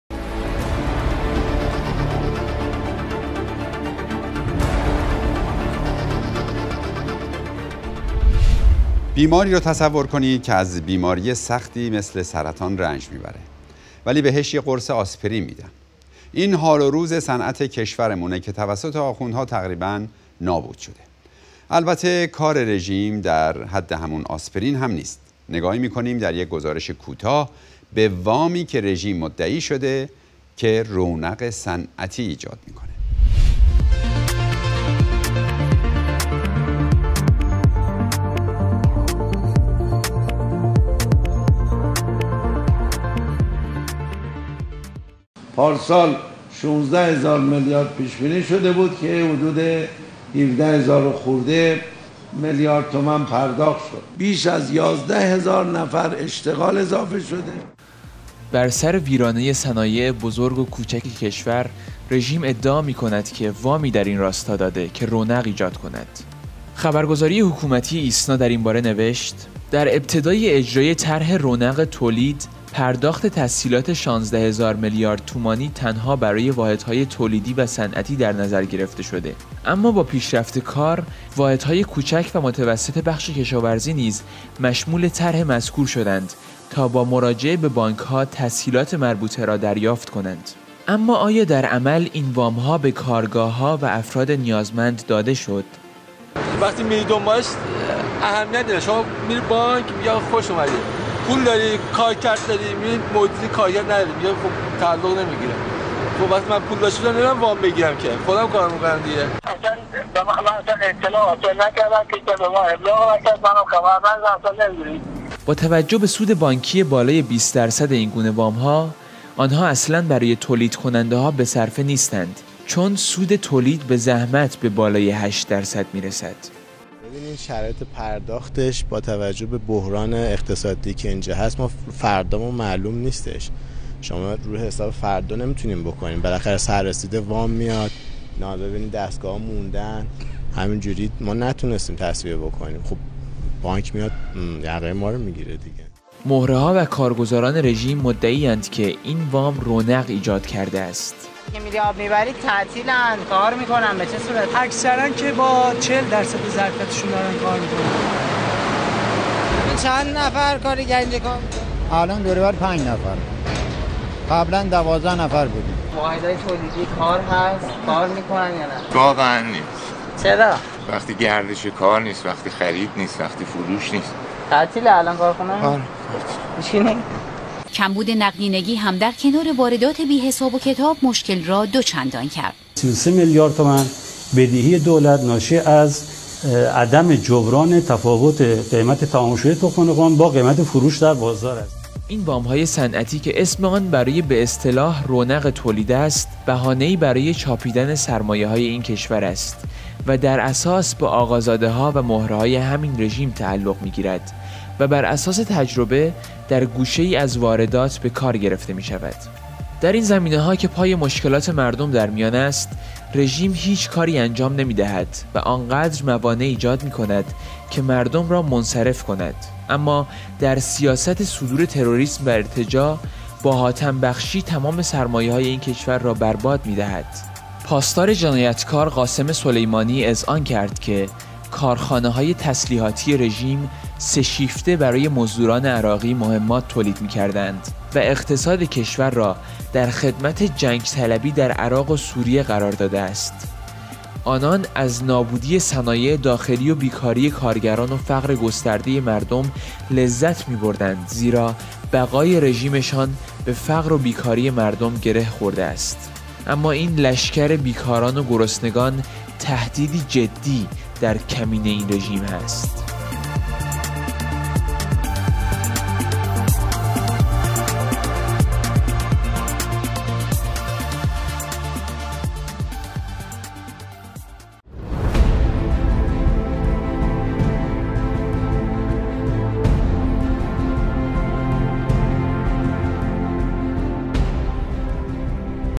وام صنعتی برای رونق اقتصادی توسط دولت روحانی
در این گزارش با اشاره به وضعیت نابسامان صنایع کشور نشان داده شده است که چگونه کمک های ناچیز دولت به صنایع کشور تحت عنوان  وام صنعتی به مثابه قرص آسپرین برای یک فرد محتضر عمل می‌کند.
سیمای آزادی تلویزیون ملی ایران ۲۵ مرداد ۱۳۹۶